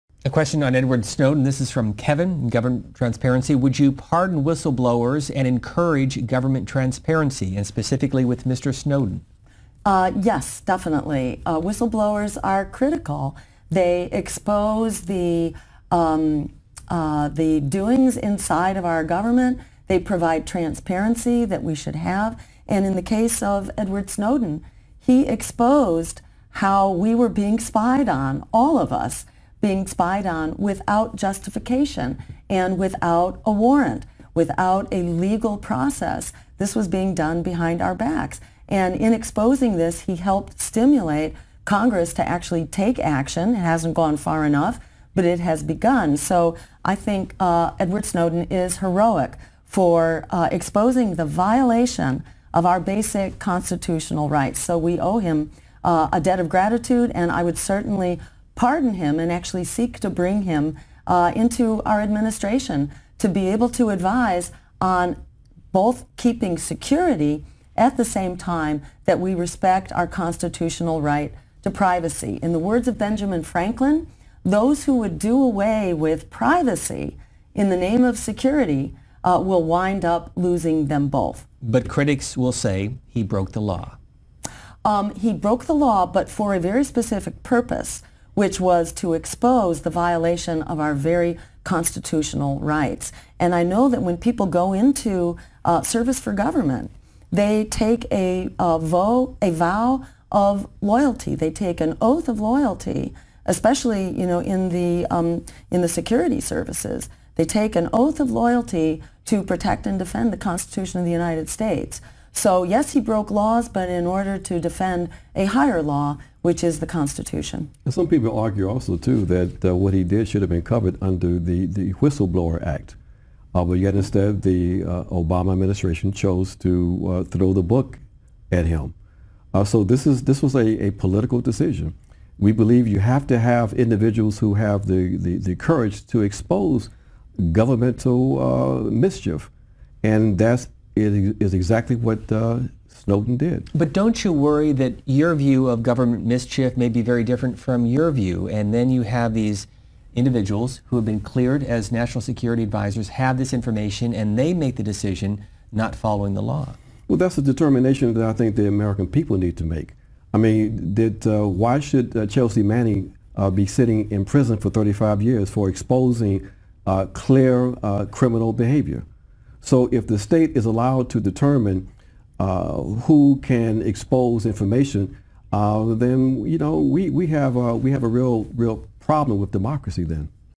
Dr. Jill Stein Would Pardon Snowden and invite him into Stein Administration
Green Party Presidential and Vice Presidential Candidates Dr. Jill Stein and Ajamu Baraka discuss pardoning Edward Snowden and inviting him into their in Administration wer they to win. C-Span interview on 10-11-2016